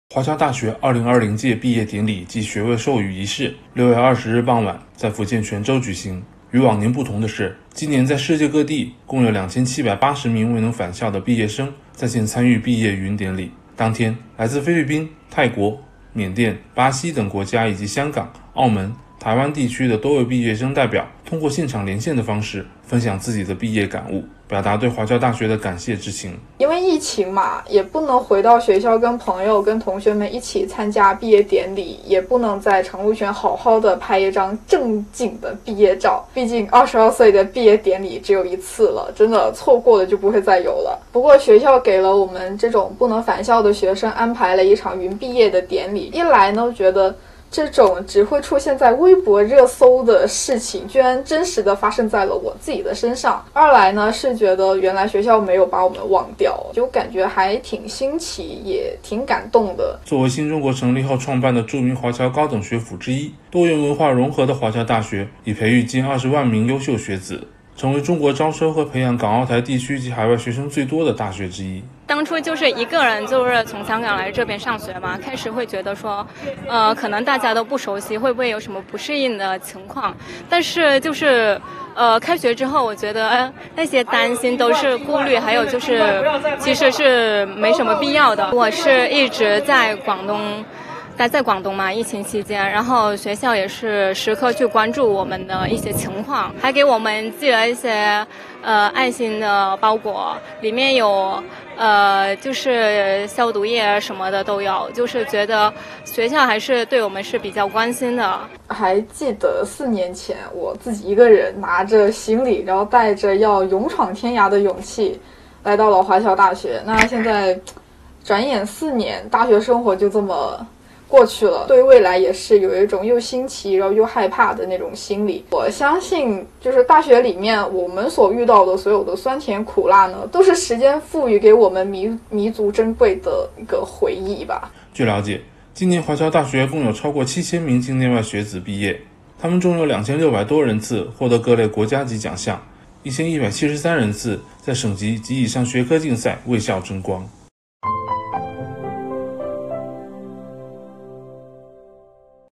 2780名境内外学子“云”参与华侨大学毕业典礼
华侨大学2020届毕业典礼暨学位授予仪式，6月20日傍晚在福建泉州举行。与往年不同的是，今年在世界各地共有2780名未能返校的毕业生在线参与毕业“云”典礼。当天，来自菲律宾、泰国、缅甸、巴西等国家以及香港、澳门、台湾地区的多位毕业生代表通过现场连线的方式，分享自己的毕业感悟。